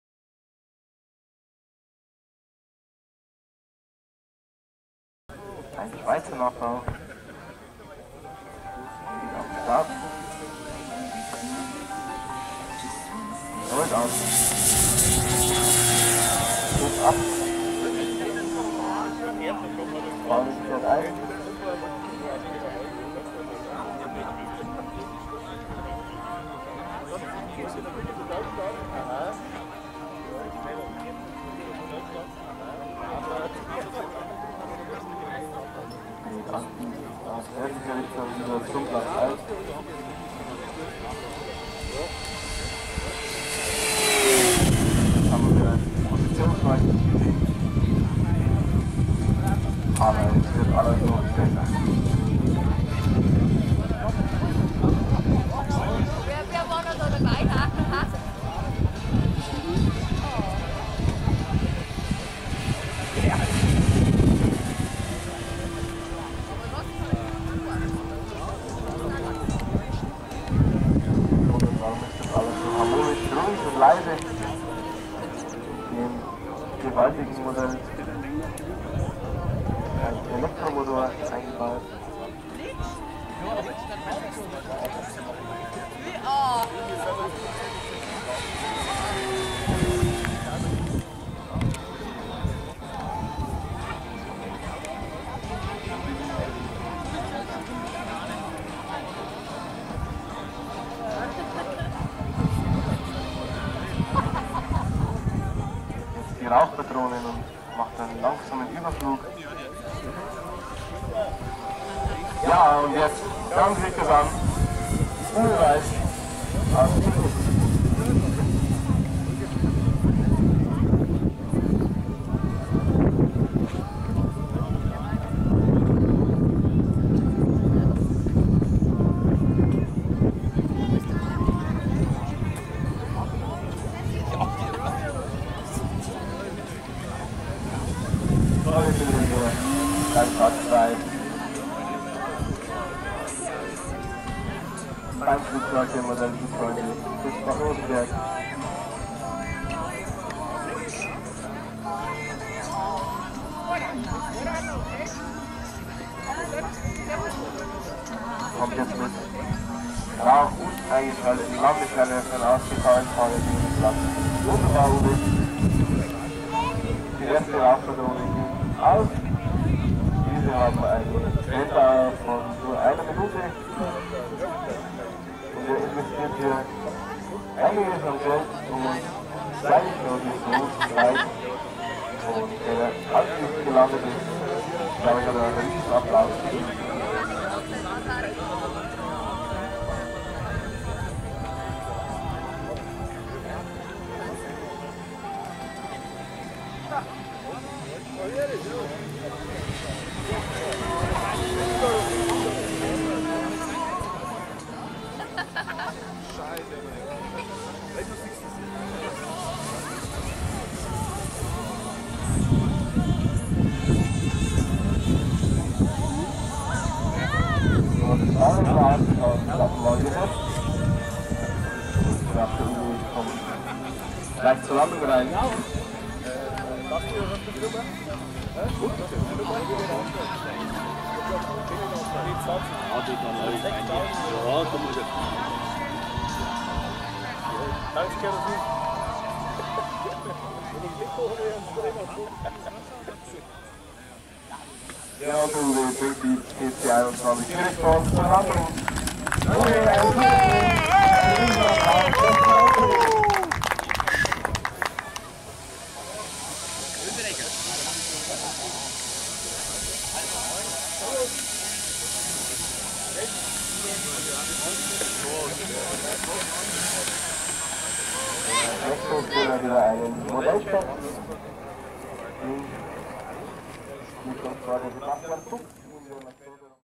Pilatus PC-21 - ESM / Lindinger - 1800mm - Hacker A60 - Flugtag 2015 Sulzbach-Rosenberg - Uwe
Flug einer Pilatus PC-21 mit Rauchpatronen von ESM / Lindinger.
Flight of an Pilatus PC-21 with smoke by ESM.

Technische Daten:
Hersteller / Kit: ESM
Spannweite / Wing Span: 1800mm (71")
Gesamtlänge / Length: 2070mm (81,5")
Fluggewicht / Flying Weight: 8,5 kg
Motor / Engine: Hacker A60
Regler: 80A
Akku: 11S/5000mAh
Propeller: 3-Blatt Ramoser 17,2"
Fahrwerk: SLR V3